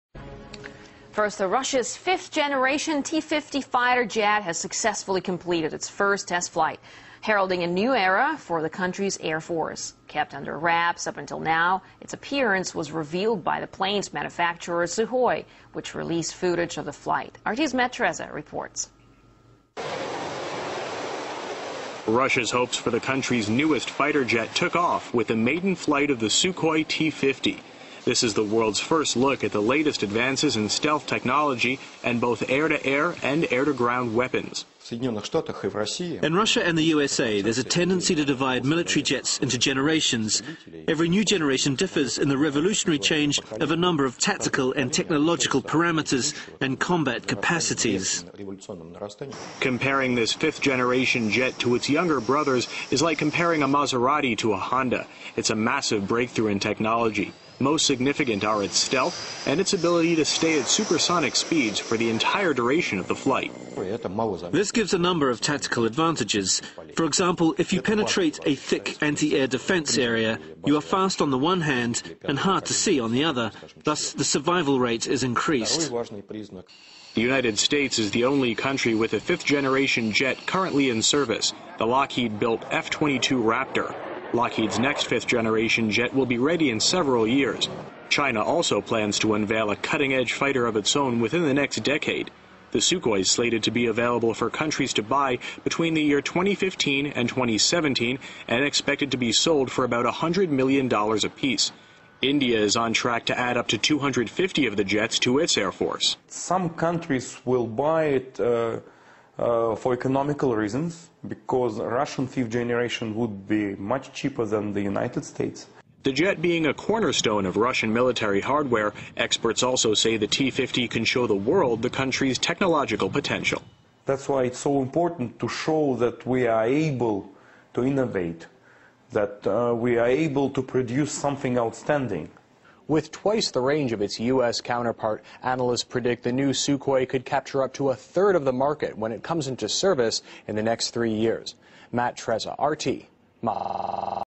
premier vol du Sukhoi T50 S/T
Pourquoi cette vidéo ? La Russie semble rattraper son retard sur les Etats Unis (qui commencent à limiter les commandes de haute technologie).
La Russie redevient un acteur important en termes de géostratégie et d'armement (voir les missiles S300 livrés à l'Iran), le Sukhoi en est une illustration.